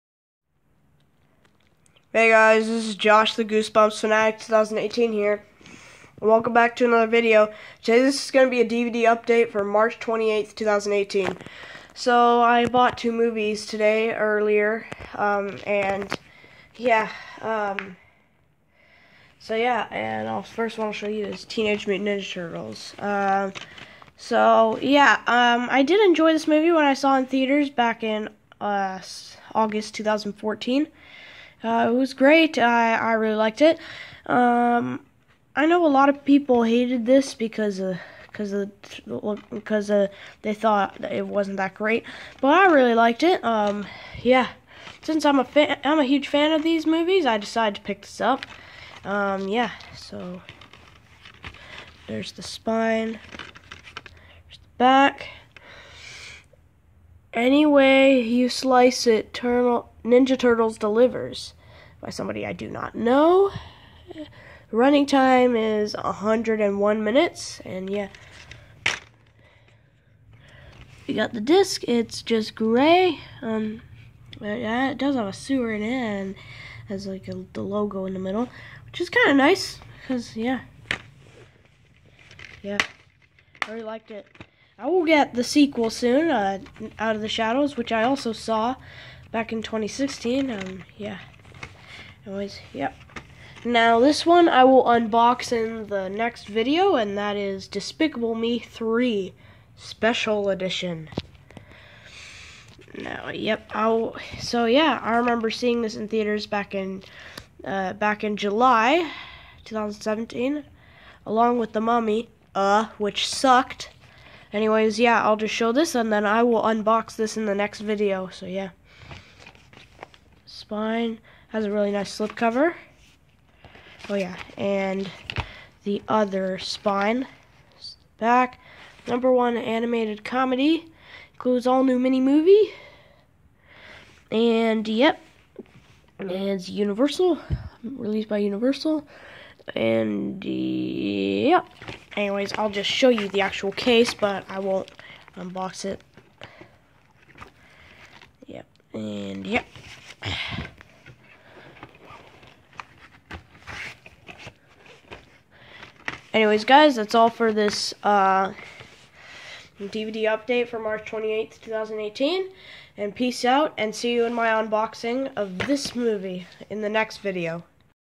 DVD Update for March 28, 2018
I went to Walmart today and got Teenage Mutant Ninja Turtles (2014) and Despicable Me 3. And I'll be unboxing Despicable Me 3 in the next video.